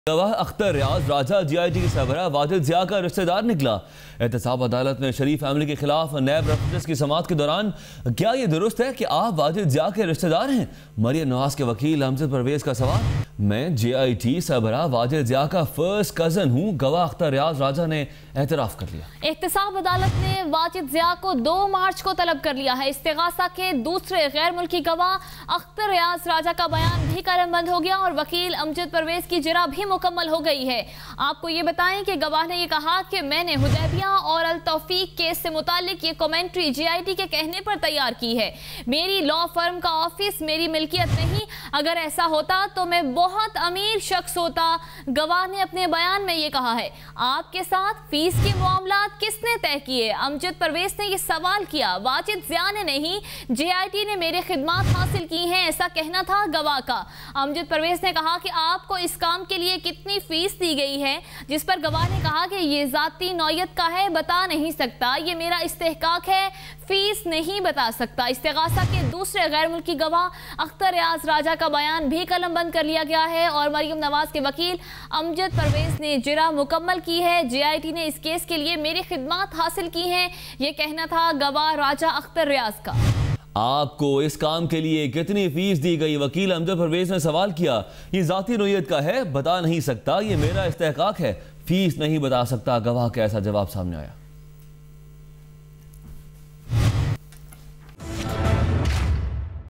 CapitalTV; Witness in NAB references against Sharif Family confesses to be first cousin of Wajid Zia
Accountability Court conducts hearing in NAB references against Sharif Family , witness Raja Akhter Riaz confesses to be first cousin of JIT head Wajid Zia, Court summons Wajid Zia on March 2nd. Watch video for more details